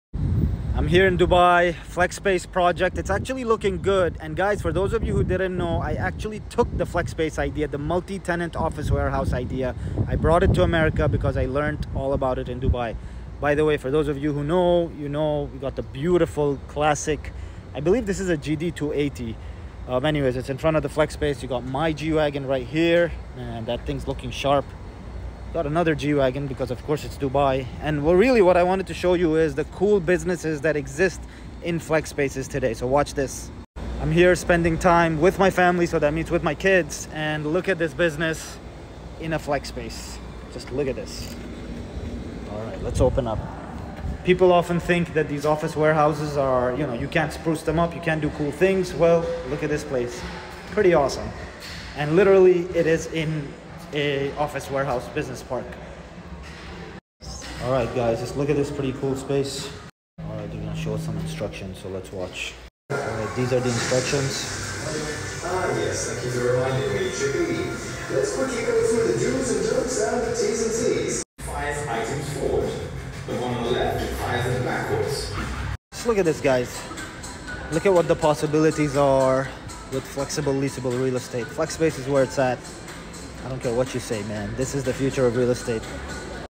Inside Dubai's Cutting-Edge Flex Spaces
Live from Dubai! Check out the incredible Chaos Karts flex space project. This multi-tenant office warehouse idea was inspired by Dubai and brought to America – and it’s thriving!

Flex spaces are revolutionizing real estate. Just look at the amazing businesses thriving in these spaces. People think you can’t make them look great, but this project proves otherwise!